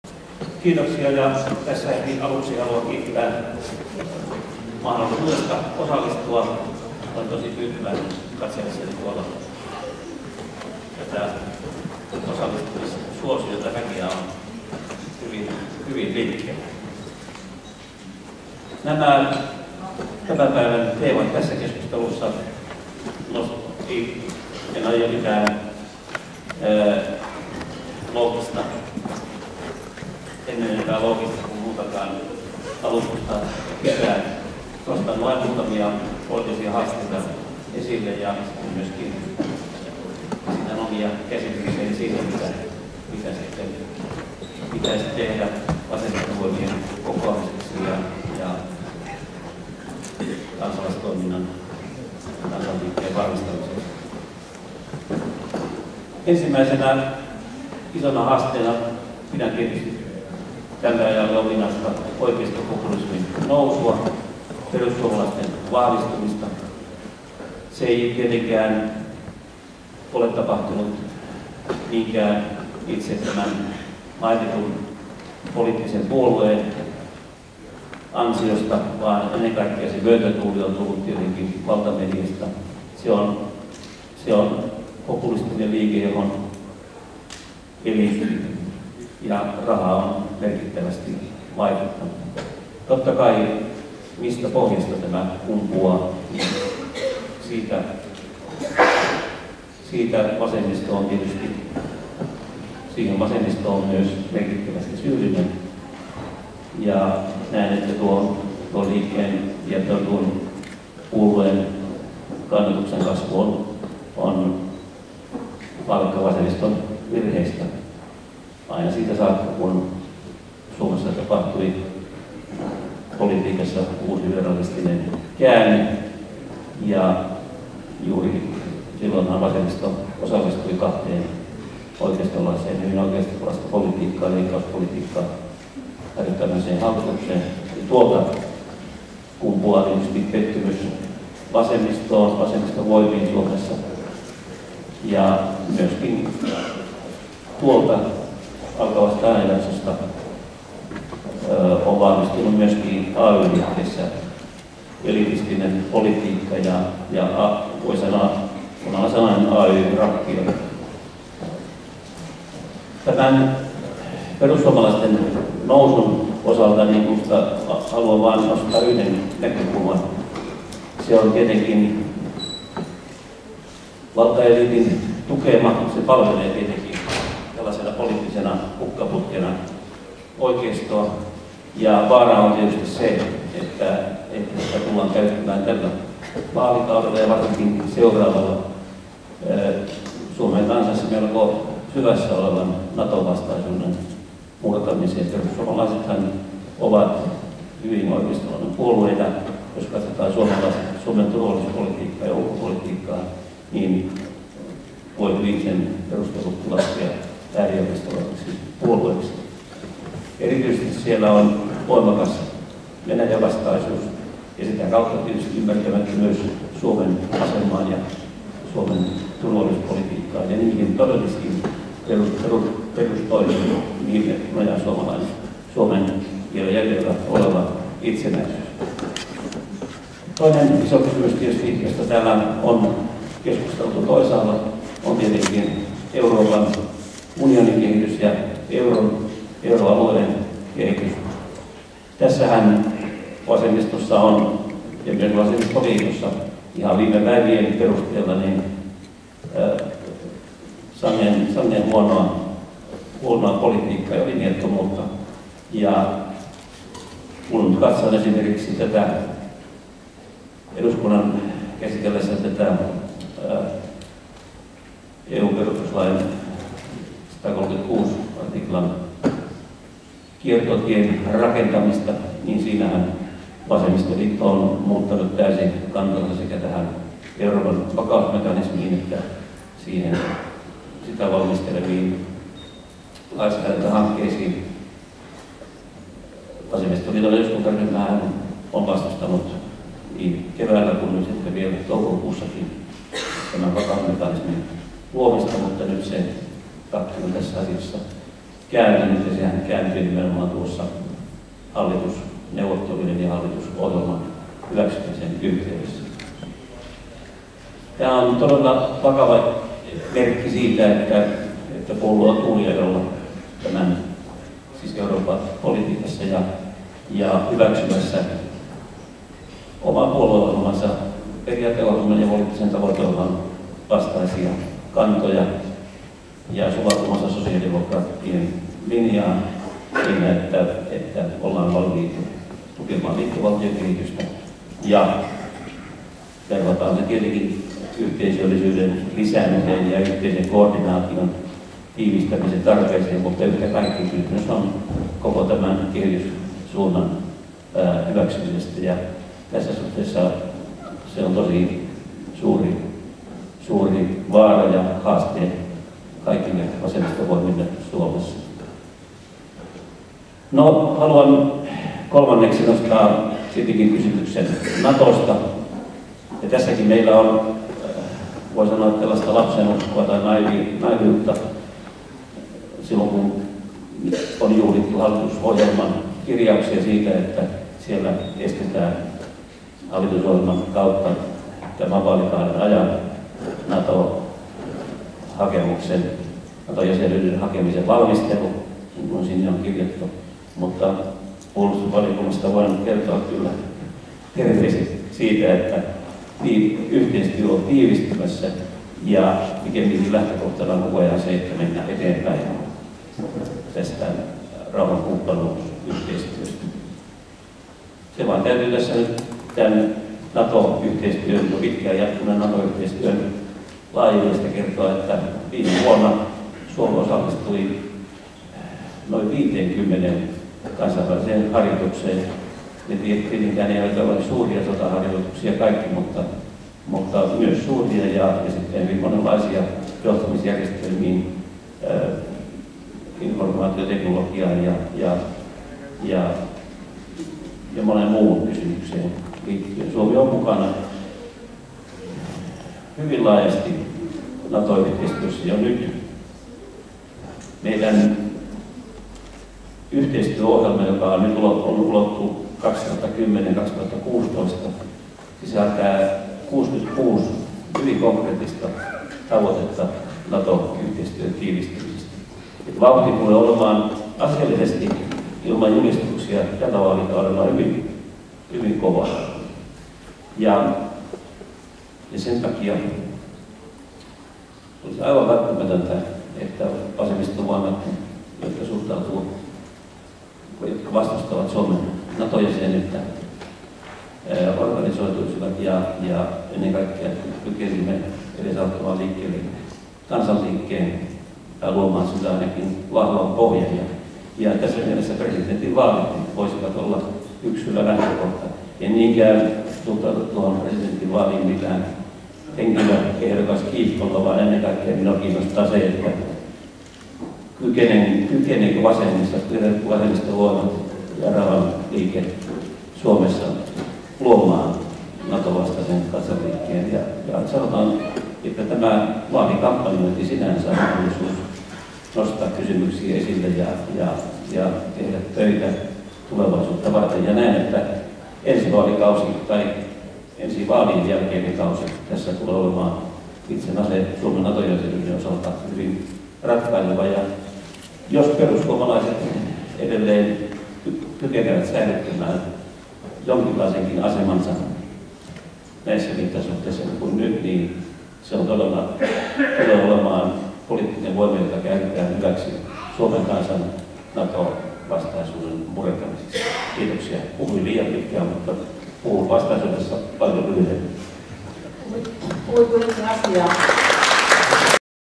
Vaihtoehtoisen vasemmiston haaste
Kansanedustaja Jyrki Yrttiaho (vas) pohtii vaihtoehtoisen vasemmiston haasteita Tiedonantaja-festivaalin paneelikeskustelussa Tampereen työväentalolla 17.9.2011. Kansanedustajan mielestä Perussuomalaisten puolueen ja sen äärioikeistolaisen siiven hälyttävä nousu on palkkaa vasemmiston tekemistä virheistä, joista olisi nyt viimeistään otettava opiksi. Vasemmistovoimien on tehtävä yhteistyötä tärkeissä kysymyksissä kuten  Naton vastaisen kansanliikkeen rakentamisessa.